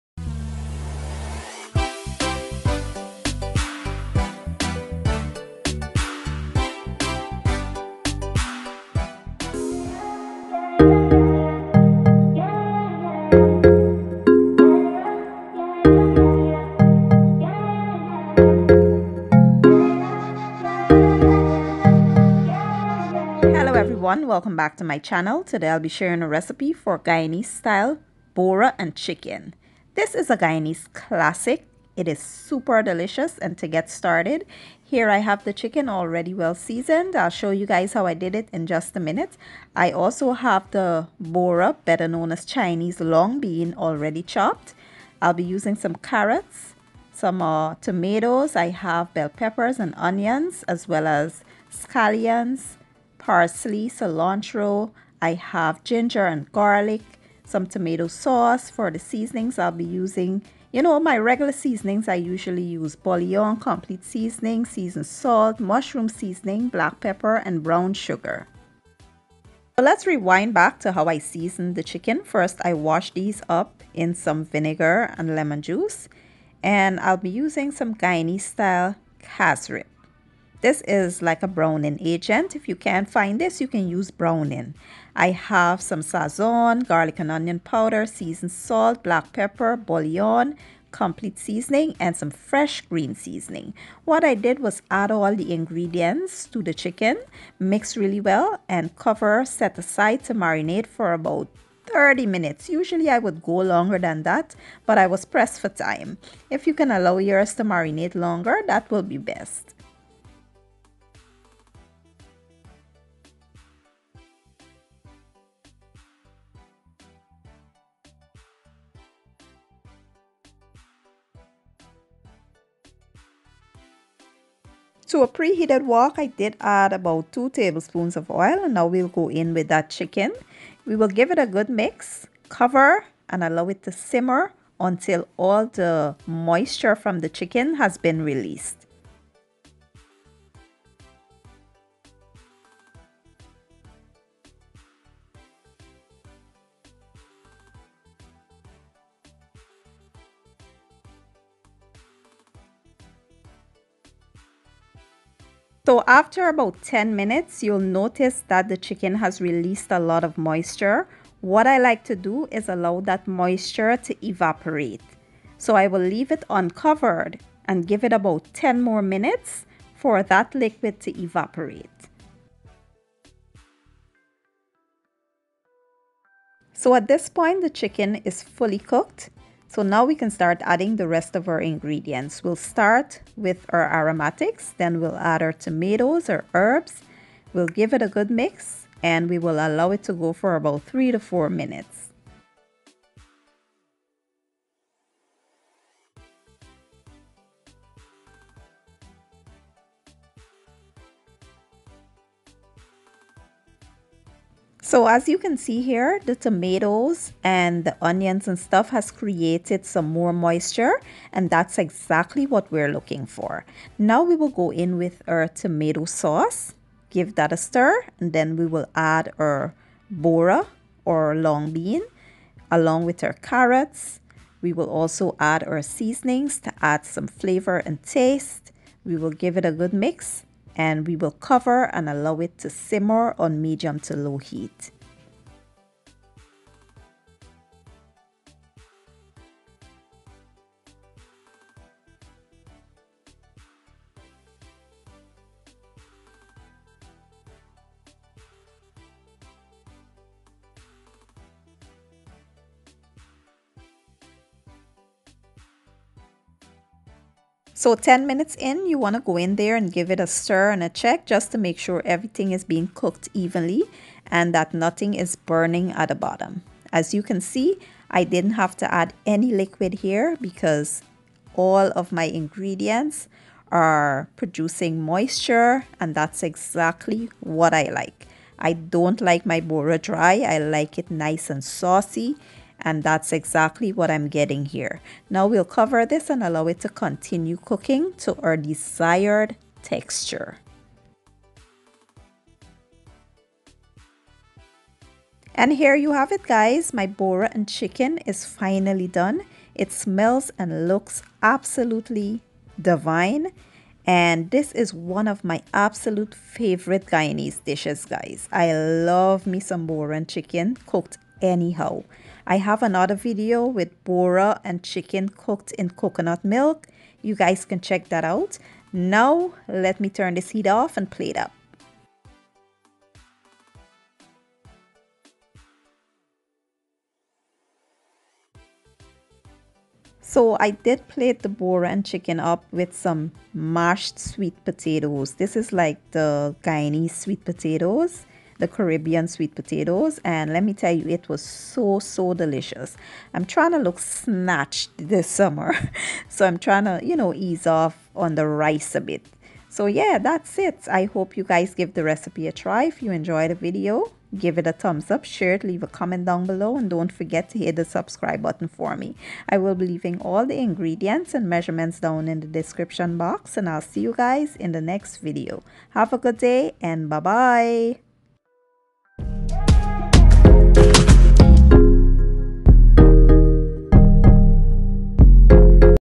Guyanese bora & chicken/ long bean | recipe
2 lbs chicken
2 tbsp green seasoning
2 tbsp chicken bouillon
2 tsp complete seasoning
1 tsp seasoned salt
1 tbsp garlic powder
1 tbsp onion powder
1 pack sazon
1/2 tsp black pepper
1 tbsp cassareep or browning

6  cups Chopped bora/long bean
1 large carrot Chopped
1 medium onion Chopped
2 Roma tomatoes Chopped
8 cloves garlic Chopped
1 cup Chopped bell peppers
Chopped scallions
Chopped cilantro
Chopped parsley
1/2 cup tomato sauce
2 tsp mushroom seasoning
1 tsp seasoned salt
1 tsp complete seasoning
1/2 tsp black pepper
1 tsp brown sugar